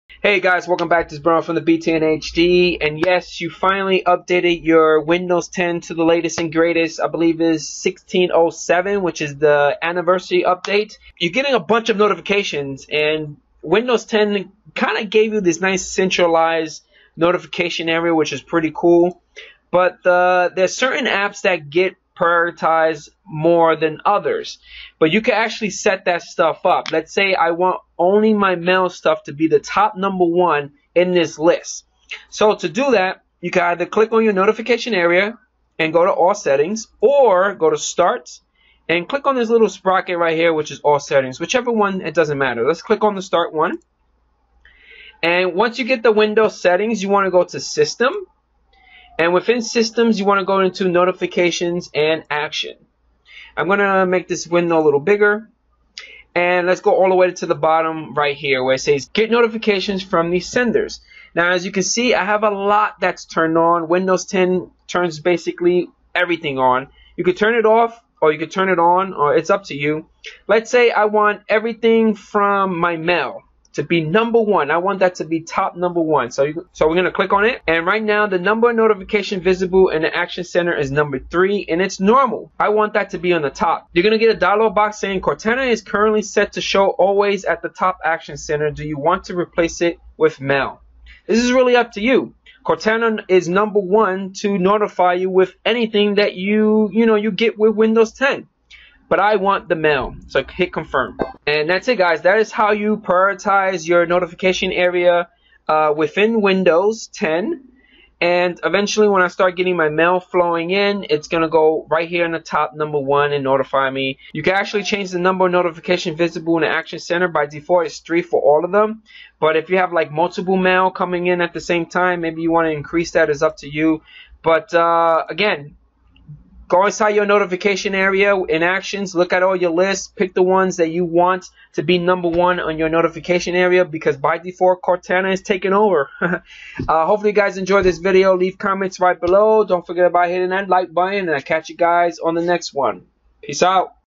Prioritize Notifications in Windows 10 Action Center!
Showing you guys how to prioritize notifications within Windows 10 Anniversary update (Version 1607). Keep it clean within the Action Center, so you can get that latest updates to your favorite apps first.